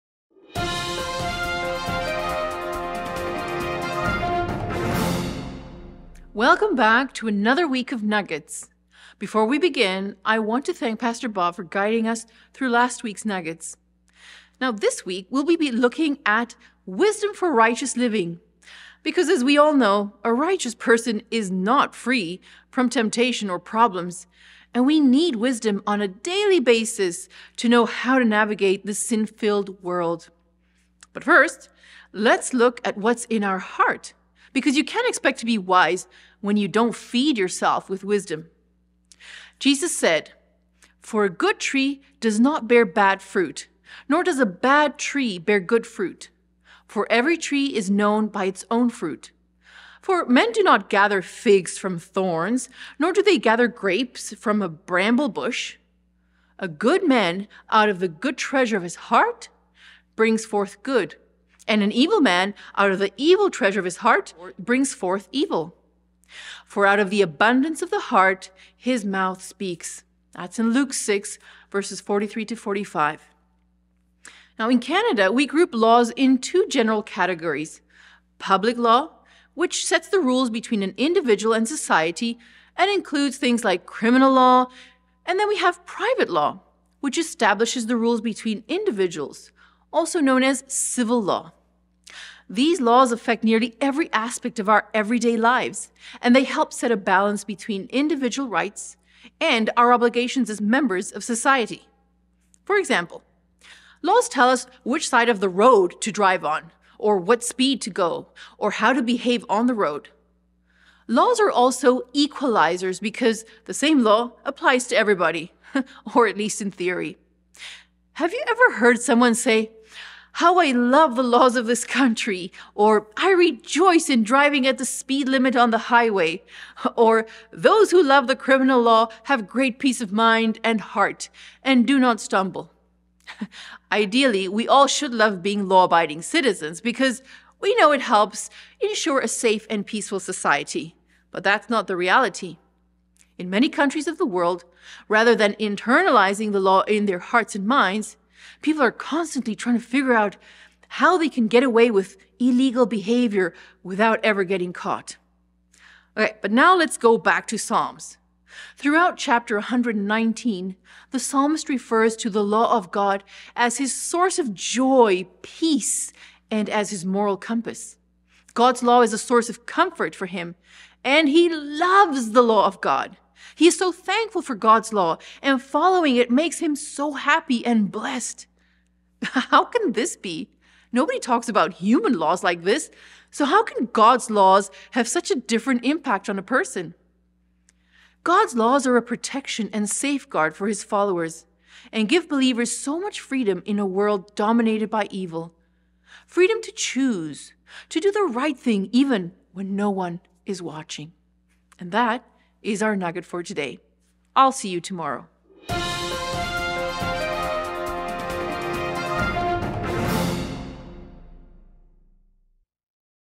Lesson 8 - Sat.: Wisdom for Righteous Living. & Sun.: Your Word I Have Hidden in My Heart.
Throughout chapter 119, the psalmist refers to the law of God as his
source of joy, peace, as his moral compass. How can God’s laws have such a
 different impact on a person?